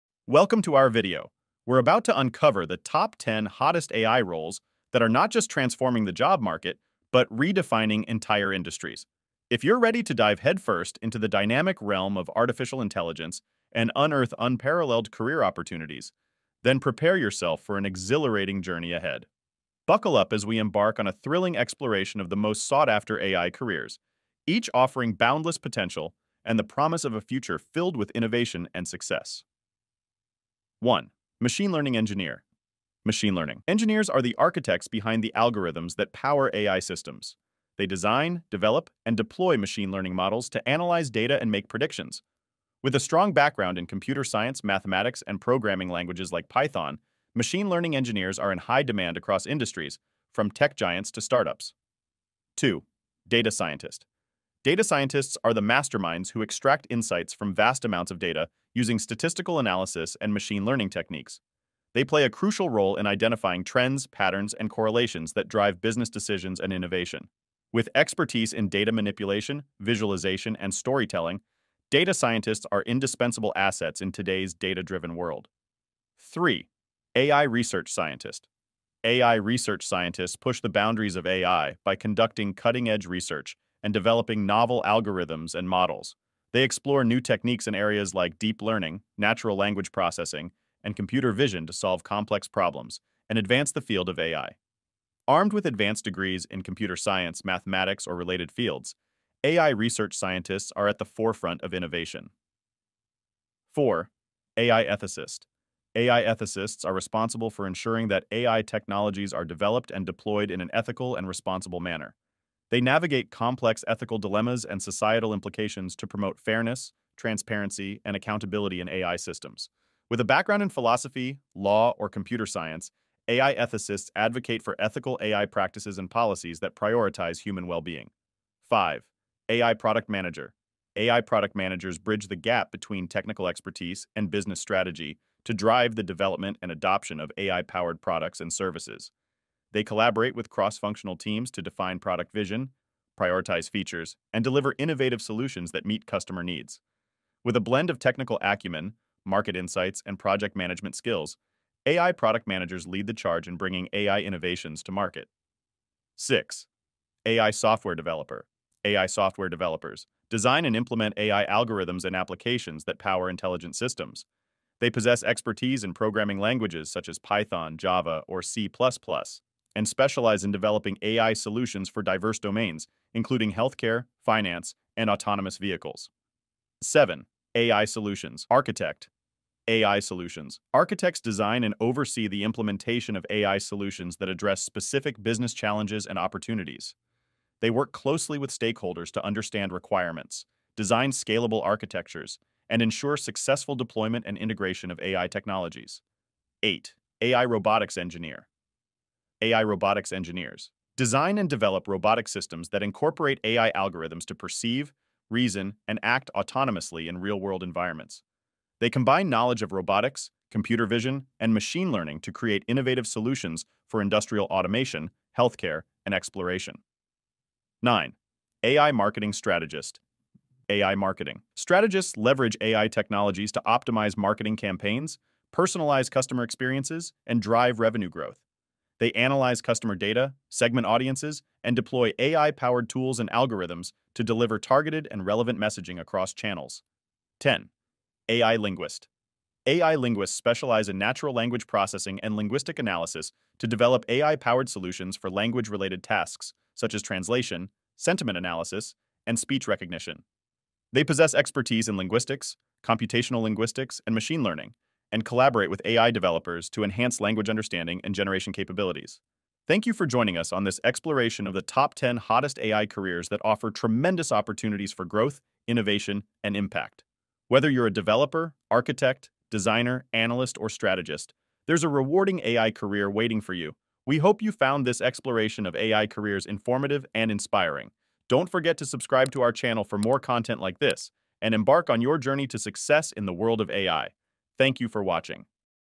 Best AI Jobs: Top 10 AI Careers for 2024
In this video, we unveil the most in-demand AI roles, offering tremendous opportunities for growth, innovation, and impact. Discover how these careers are shaping the future.

Explore diverse and exciting roles such as AI Engineer 👨‍💻, Data Scientist 📊, AI Research Scientist 🔬, AI Ethicist 🤖, and AI Solutions Architect 🏗️. Whether you're a professional seeking career advancement or a newcomer eager to enter the industry, these positions offer immense potential for growth and innovation. Join us as we delve into each role, discussing key responsibilities and contributions to provide valuable insights for anyone interested in pursuing a career in AI.

With the demand for AI talent skyrocketing across industries, now is the perfect time to explore these dynamic career paths and position yourself for success in the age of AI. 🌐 Don't miss out on this opportunity to discover the endless possibilities awaiting you in the world of artificial intelligence. 🚀